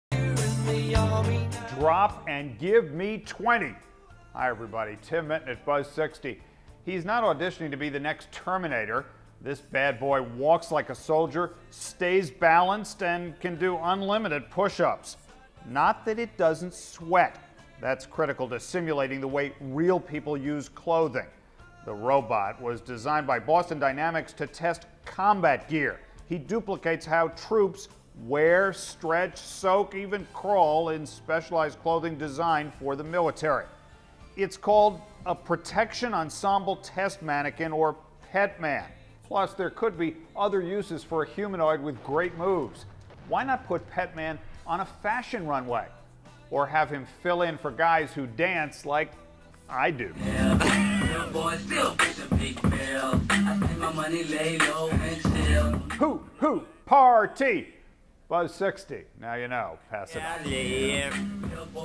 PETMAN Robot Walks, Sweats Like a Human
The latest in robot technology is PETMAN, an anthropomorphic prototype developed by Boston Dynamics. The developer is best known for its videos of the Big Dog robotic animal. PETMAN walks, does push-ups, even sweats.  Currently, it's being used to test clothing for the U.S. military. Tim Minton found some other possible uses.